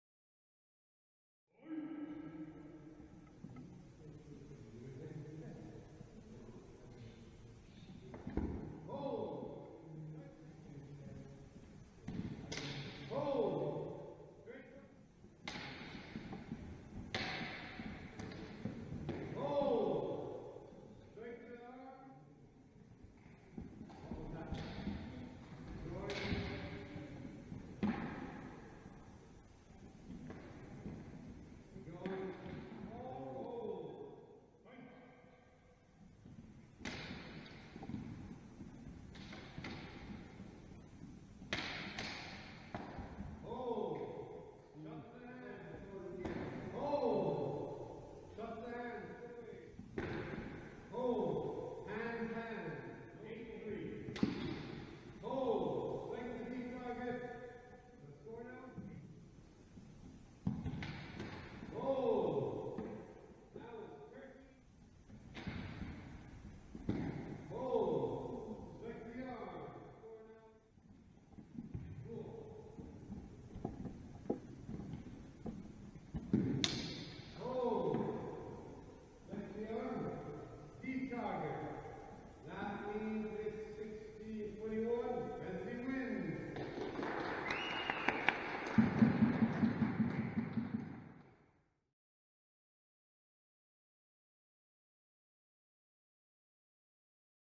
2022 Scholar Exam Day - first bouts: Michelangelo vs Wesley
2022 German Longsword Course Group
Scholar Exam Day - first bouts